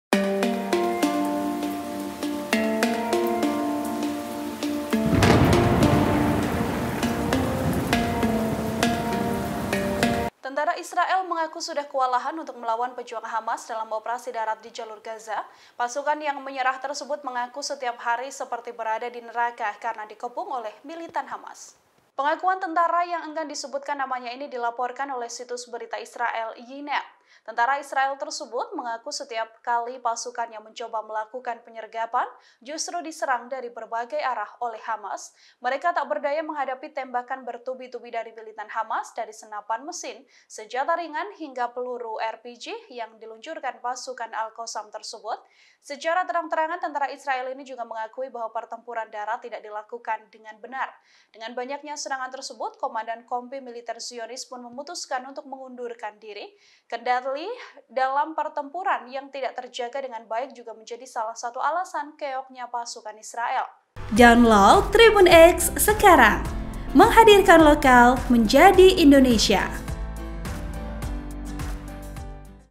PENGAKUAN Pasukan Israel Kewalahan Melawan Hamas: Setiap Hari Kita seperti Ada di Neraka
TRIBUN-VIDEO.COM - Tentara Israel mengaku sudah kewalahan melawan pejuang Hamas dalam operasi darat di jalur Gaza.

Pasukan yang menyerah tersebut mengaku siap hari seperti di neraka, karena dikepung oleh militan Hamas.

Pengakuan tentara yang enggan disebut namanya itu dilaporkan oleh situs berita Israel, Ynet.